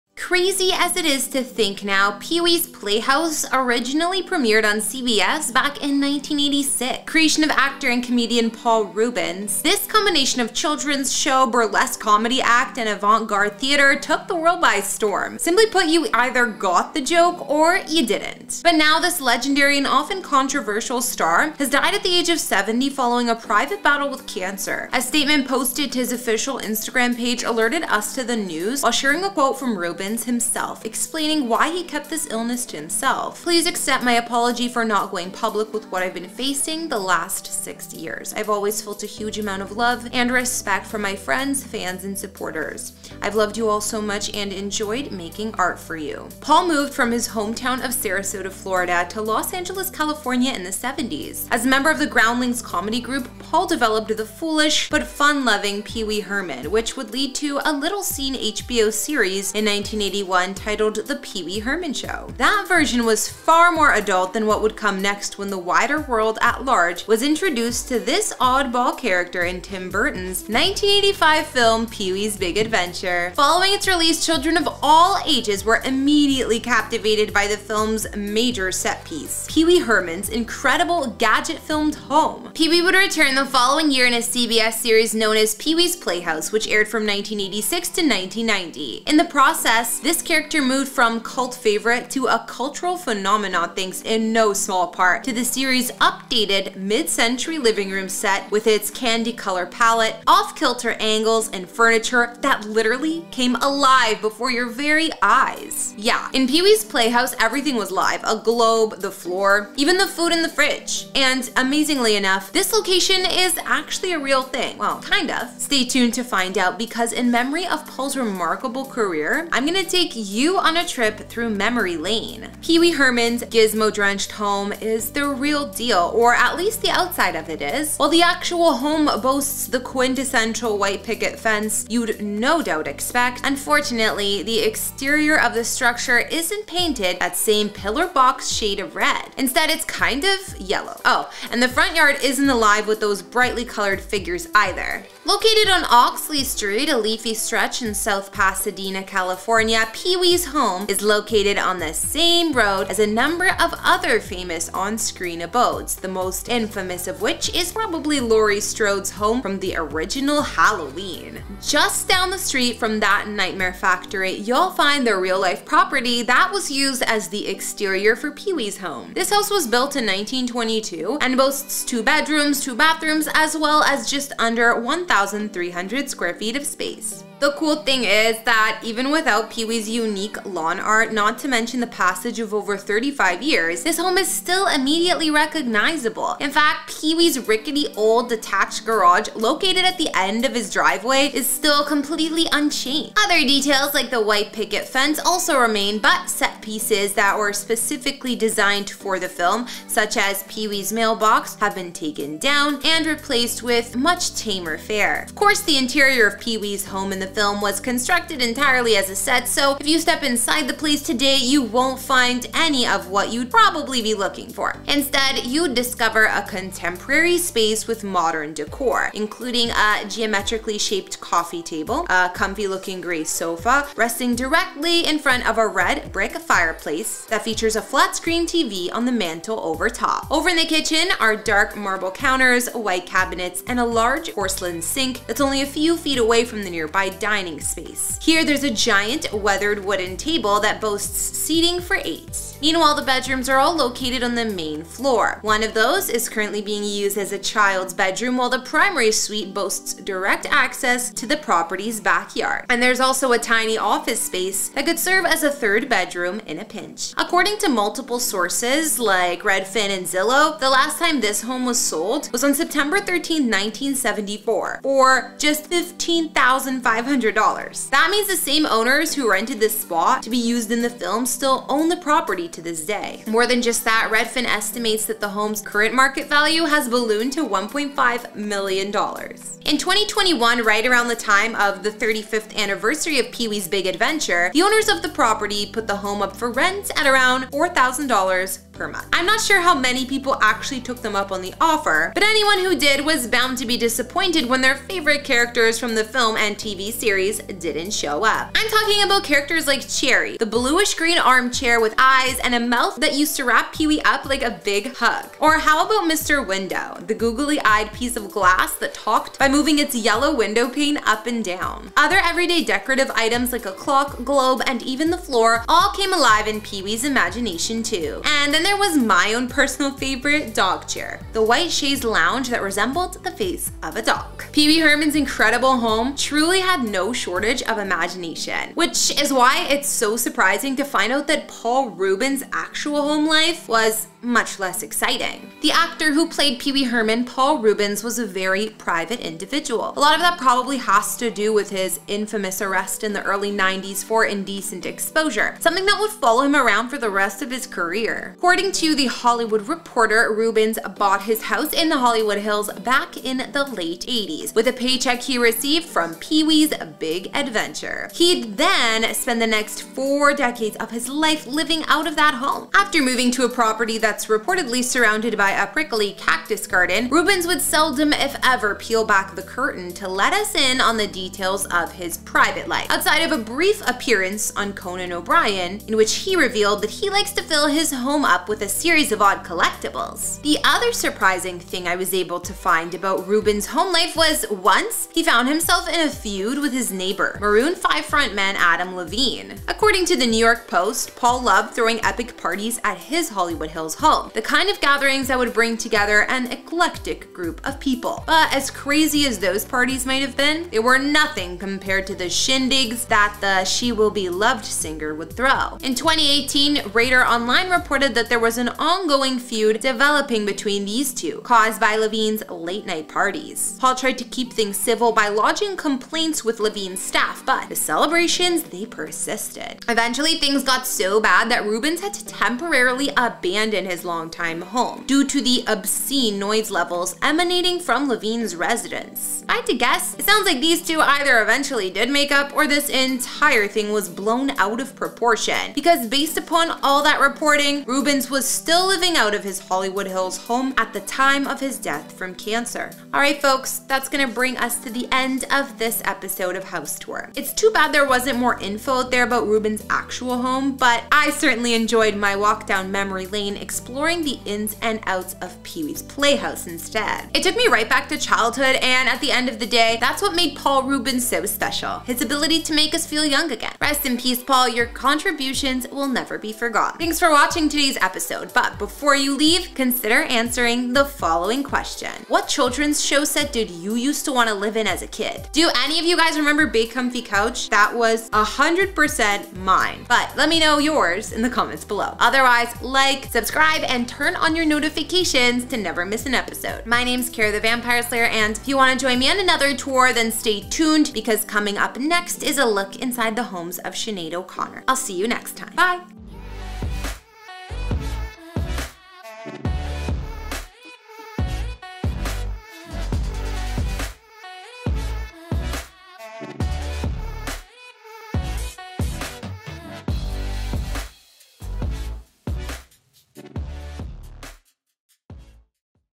Paul Reubens | House Tour | Inside Pee Wee's Playhouse and Paul's Million-Dollar Home
Crazy as it is to think now, “Pee-Wee’s Playhouse” originally premiered on CBS back in 1986. The creation of actor and comedian, Paul Reubens, this combination of children’s show, burlesque comedy act, and avant-garde theater took the world by storm. But now, this legendary – and often controversial – star has died at the age of 70 following a private battle with cancer. Paul moved from his hometown of Sarasota, Florida to Los Angeles, California in the 70s. As a member of The Groundlings comedy group, Paul developed the foolish but fun-loving Pee Wee Herman which would lead to a little-seen HBO series in 1981 titled “The Pee Wee Herman Show”. Pee Wee would return the following year in a CBS series known as “Pee-Wee’s Playhouse”, which aired from 1986 to 1990. In the process this character moved from cult favorite to a cultural phenomenon thanks in no small part to the series’ updated Mid-Century living room set with its candy-color palette, off-kilter angles and furniture that literally came alive before your very eyes!